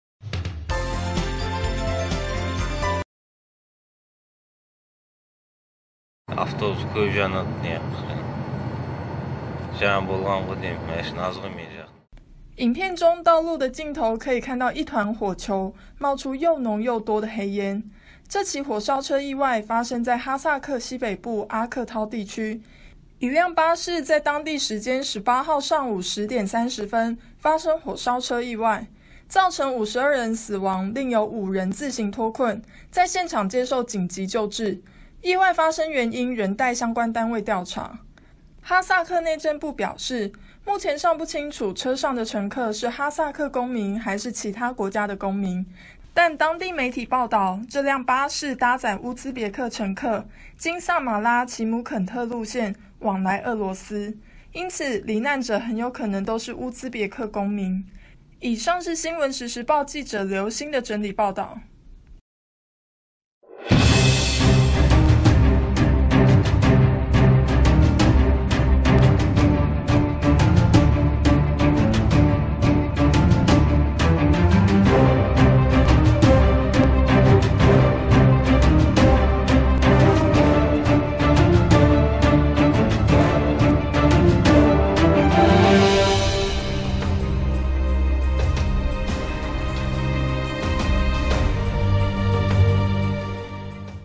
哈薩克巴士火燒車致52死，現場影片曝光（《新聞時時報》2018年1月18日）
哈薩克西北部阿克濤地區一輛巴士在當地時間18號上午10點30分，發生火燒車意外，造成52人死亡，另有5人自行脫困，在現場接受緊急救治，意外發生原因仍待相關單位調查。